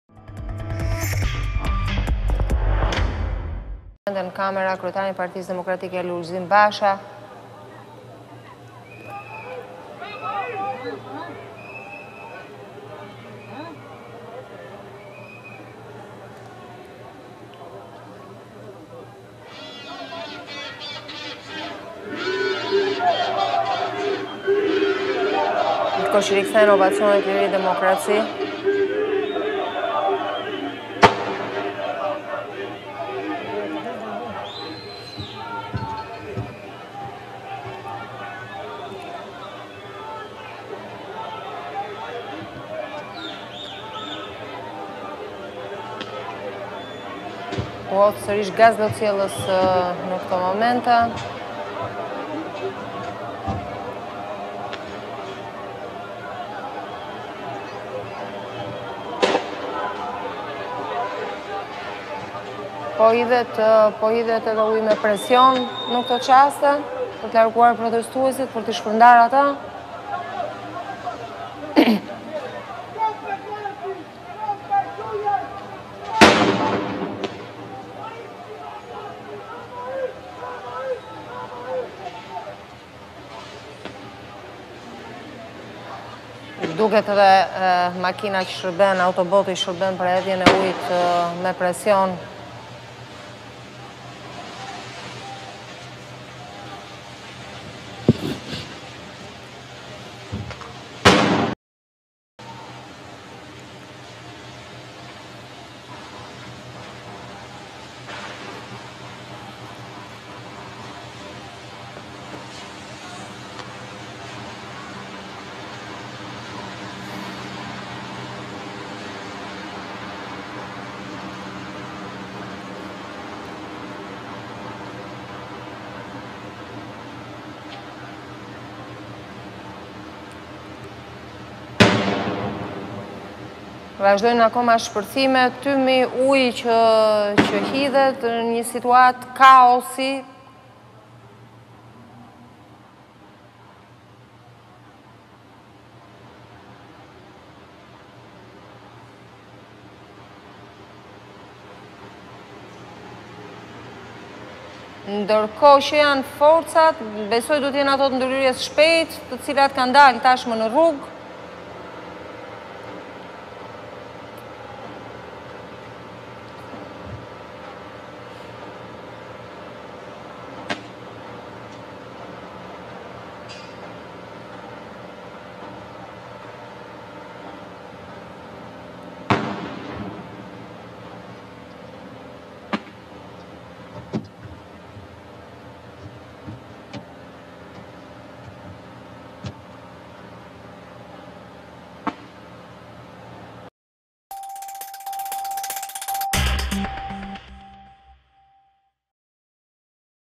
Protesta, tensione perpara Kuvendit | ABC News Albania
Pas më shumë se dy orësh qëndrim para Kuvendit, protesta e opozitës u zhvendos para Kuvendit. Aty tubimi u shoqërua me tensione.

Protestuesit hodhën bomba molotov dhe fishekzjarre në drejtim të hyrjes së Kuvendit. Nga ana tjetër policia u kundërpërgjigj duke hedhur gaz lotsjellës, çka solli edhe shpërndarjen të protestuesve.

Edhe përpara Kryeministrisë , protesta u shoqërua me tensione dhe dhunë. Tubuesit hodhën molotov dhe flakadanë drejt godinës.

Për pasojë mori flakë dera hyrëse e saj. Gjithashtu, pati përleshje mes protestuesve dhe policisë, ku u shkatërrua kordoni metalik që rrethonte kryeministrinë./abcnews.al